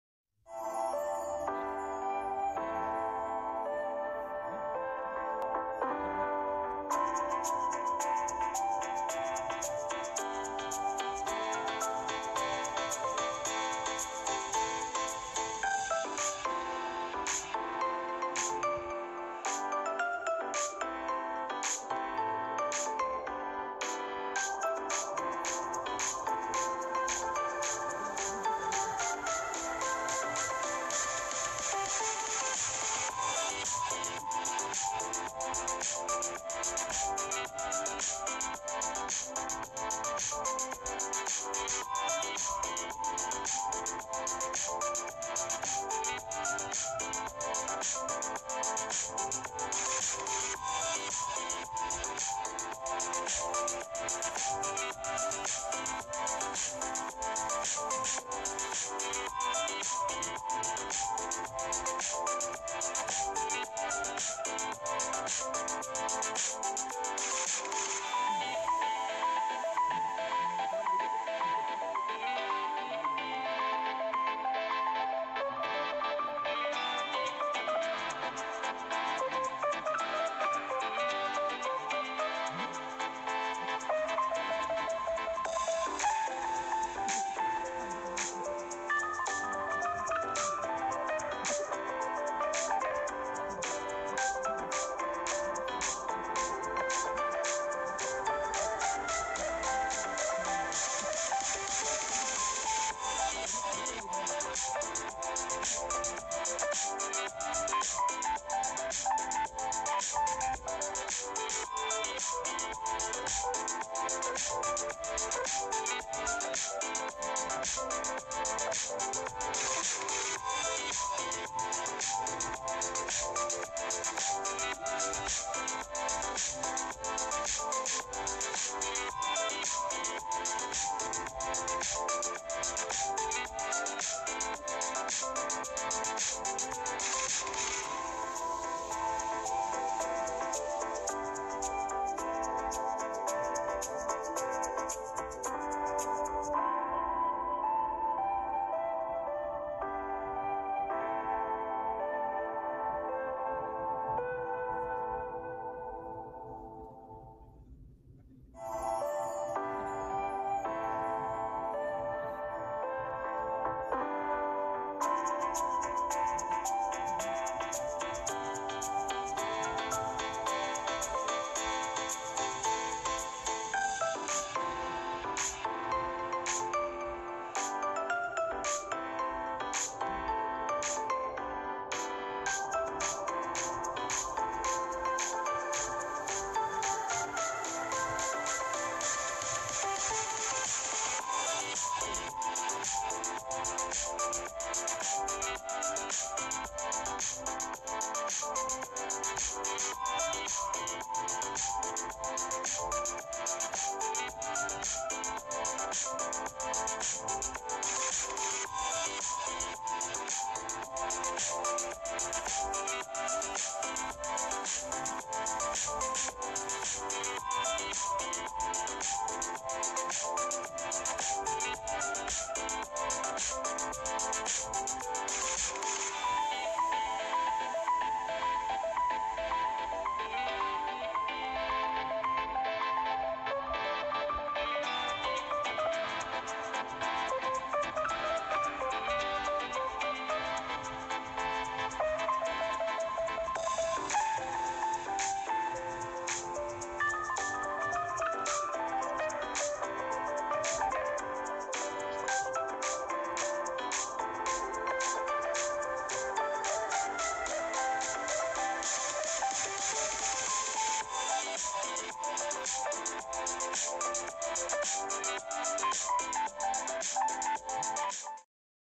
Stabery hunting Izu fruit park Hakone
Stabery Hunting failed atrnwd due to covid-19